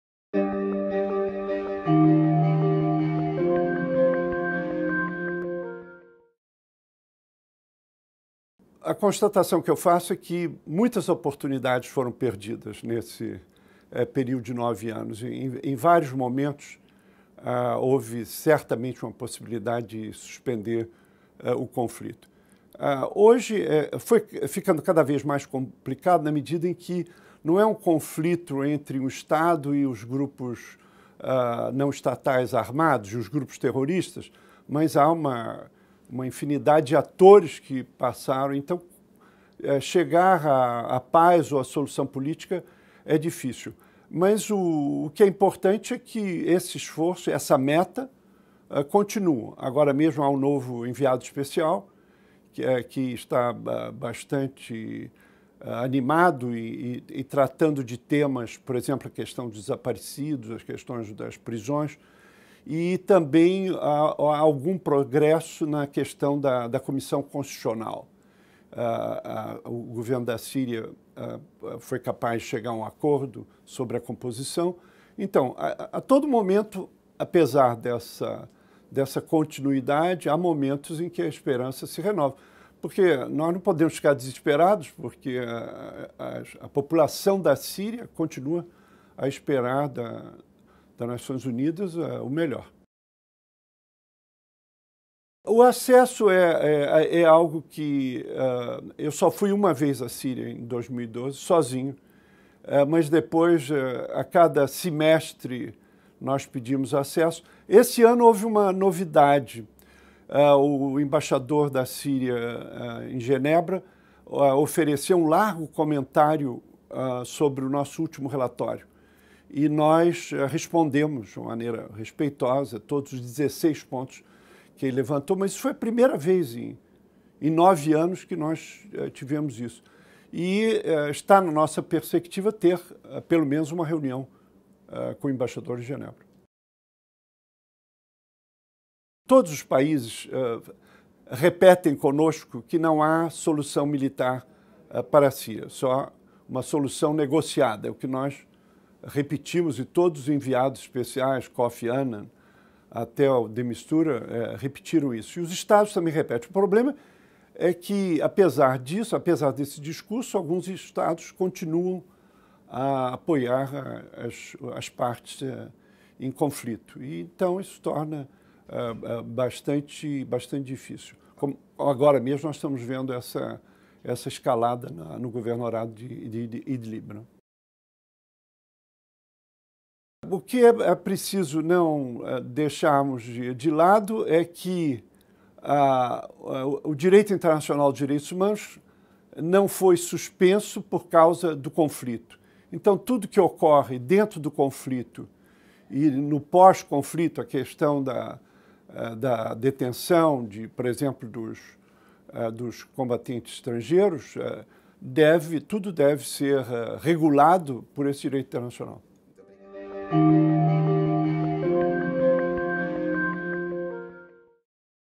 Síria: representante brasileiro fala em “ondas de esperança”
Em entrevista à ONU News, em Nova Iorque, o brasileiro Paulo Sérgio Pinheiro, presidente da Comissão de Inquérito Internacional sobre a Síria, disse haver “ondas de esperança” em prol do fim do conflito, que devem ser apoiadas por ações dos países-membros das Nações Unidas.

Nesta semana (9), a Comissão apresentou um informe ao Conselho de Segurança sobre o nono ano de atuação do grupo.